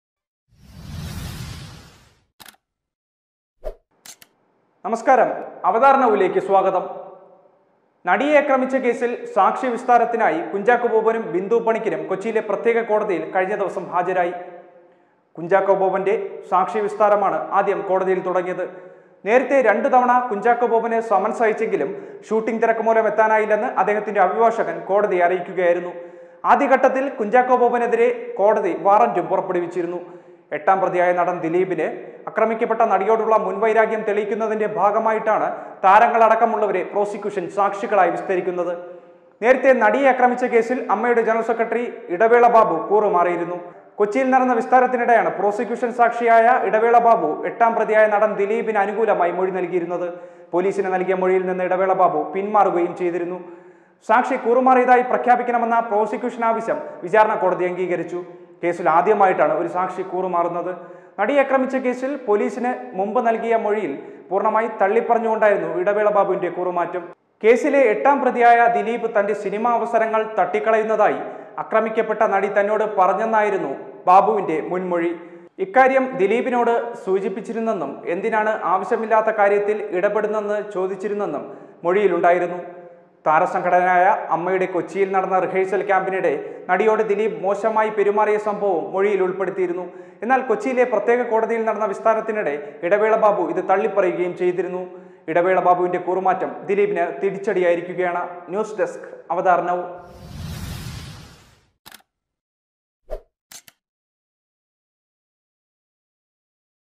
കൂറ് ദിലീപിനോട് തന്നെ ബിന്ദുപണിക്കരും കാലുമാറി | DILEEP CASE | Actress abduction case
നടിയെ ആക്രമിച്ച കേസിൽ സാക്ഷി വിസ്താരത്തിനായി കുഞ്ചാക്കോ ബോബനും ബിന്ദു പണിക്കരും കൊച്ചിയിലെ പ്രത്യേക കോടതിയിൽ കഴിഞ്ഞ ദിവസം ഹാജരായി. കുഞ്ചാക്കോ ബോബന്‍റെ സാക്ഷിവിസ്താരമാണ് ആദ്യം കോടതിയിൽ തുടങ്ങിയത്. നേരത്തെ രണ്ടുതവണ കുഞ്ചാക്കോ ബോബന് സമൻസ് അയച്ചെങ്കിലും ഷൂട്ടിംഗ് തിരക്കുമൂലം എത്താനായില്ലെന്ന് അദ്ദേഹത്തിന്റെ അഭിഭാഷകൻ കോടതിയെ അറിയിക്കുകയായിരുന്നു